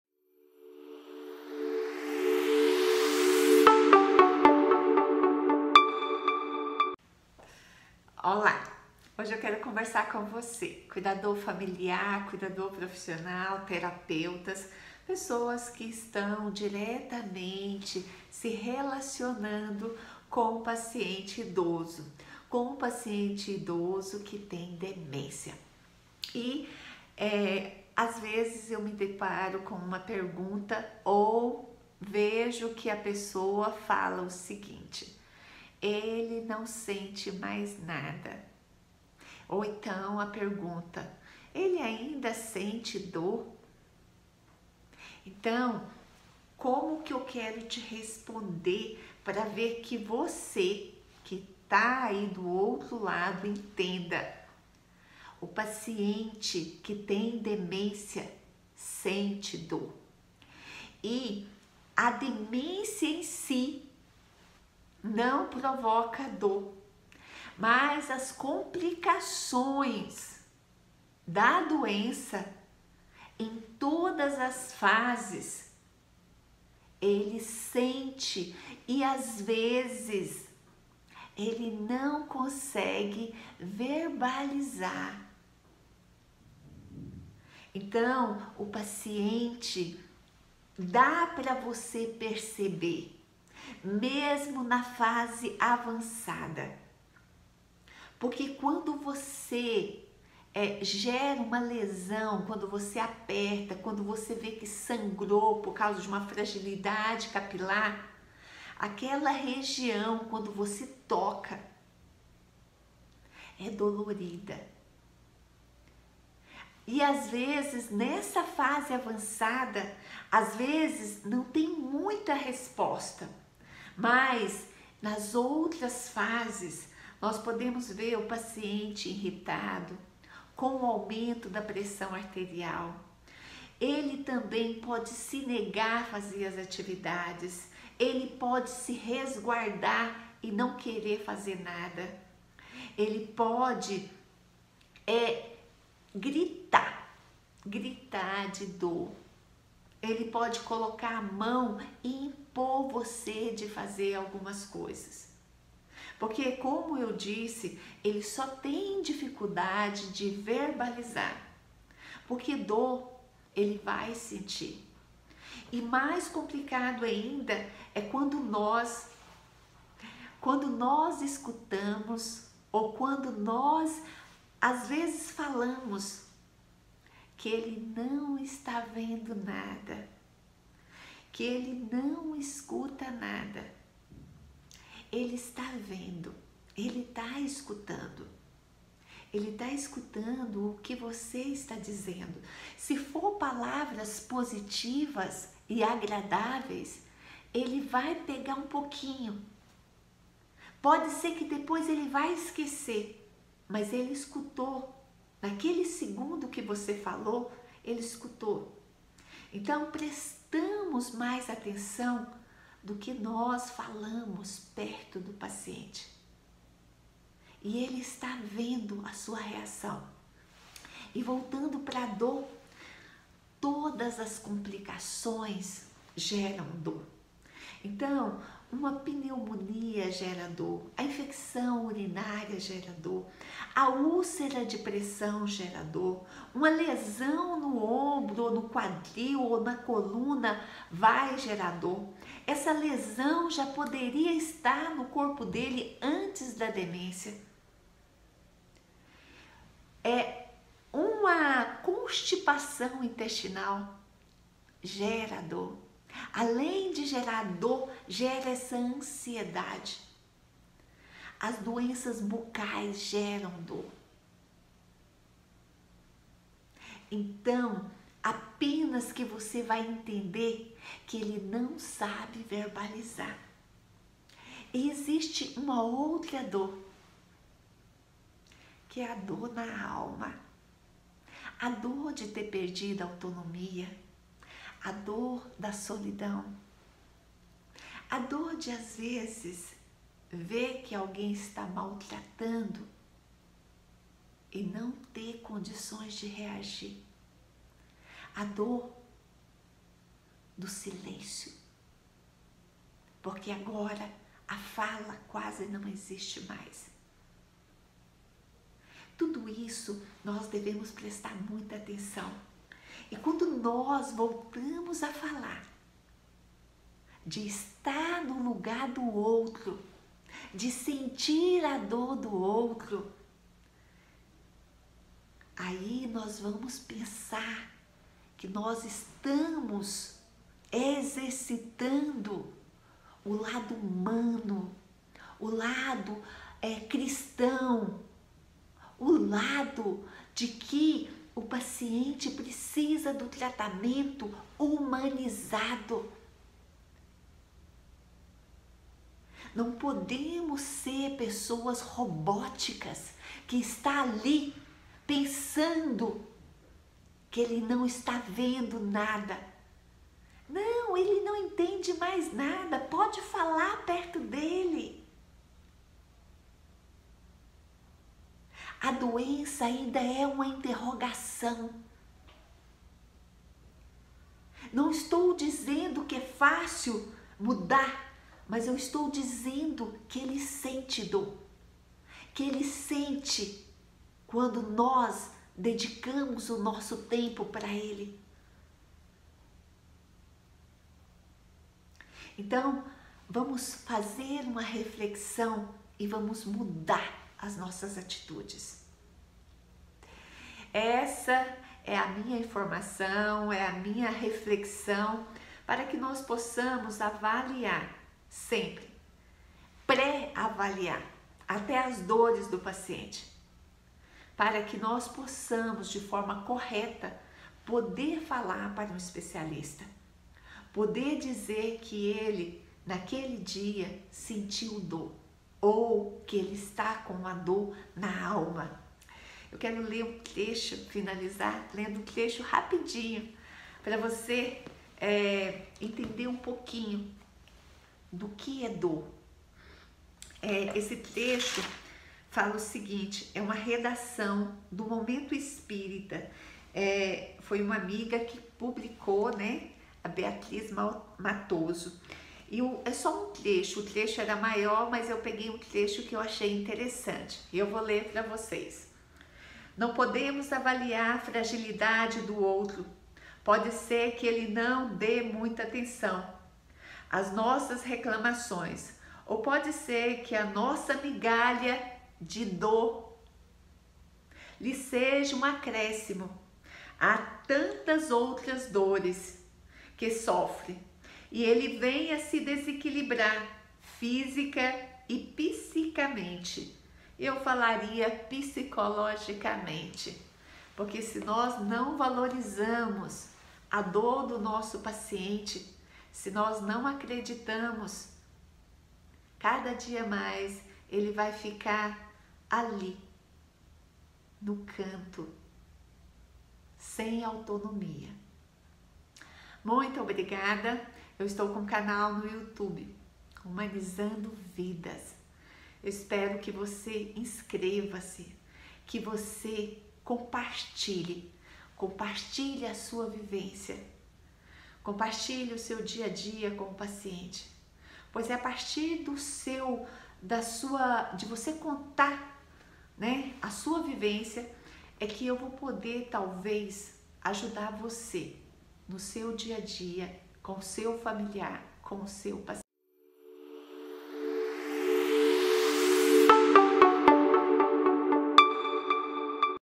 A DEMÊNCIA PROVOCA DOR??? @ ALZHEIMER
A dor é uma resposta importante do nosso corpo geralmente a qualquer estímulo doloroso ou processo inflamatório. É uma resposta do sistema nervoso que muitas vezes leva o indivíduo a uma reação de retirada ou proteção. Ela pode ser aguda, sub aguda e crônica. Leve, moderada e grave. Contínua, superficial e profunda.  Os pacientes idosos com demência em todas as fases da doença sentem dor. Eles podem ter dificuldade de localizar ou verbalizar o local exato da dor, mas Sentem dor, escutam e enxergam nossas ações para com ele.
As demências não geram dor, mas as COMPLICAÇÕES das demências provocam dores horríveis. Algumas das principais complicações são: úlceras de pressão, cárie, contraturas e rigidez, constipação intestinal, hemorroida, infecções urinaria e pulmonar, temperatura muito baixa, fragilidade capilar, artrite reumatoide, artrose, herpes zoster, calos, unhas encravadas. E o paciente pode sentir uma dor na alma por sentir se esquecido e ou na solidão.
Os estudos realizados com Ressonância Magnética revelaram que as pessoas com a Demência de Alzheimer mostram que atividade neuronal é a mesma dos indivíduos SEM  a demência de Alzheimer.
É fácil reconhecer quando o paciente idoso com demência tem dor, geralmente muda a expressão facial, ao toque pode estar quente, vermelho e dolorido, podem ou não gemer, não quer fazer movimentos que envolvam locomoção, não permite que o movimento seja realizado em toda a sua extensão, pode adotar uma postura de resguardo, podem gritar, chorar, se a dor é muscular, articular, ligamentar ou óssea pode inclinar para um dos lados como forma de proteção.
Se tiver como prevenir para que o paciente com demência não tenha o processo doloroso iniciado, há probabilidade de que as complicações sejam menores, assim sendo o paciente vai ter mais qualidade e dignidade de vida.
A dor persistente pode reduzir a mobilidade, a flexibilidade, a força muscular que leva o indivíduo pouco a pouco a ter limitações para as atividades de vida diária e também sua autonomia.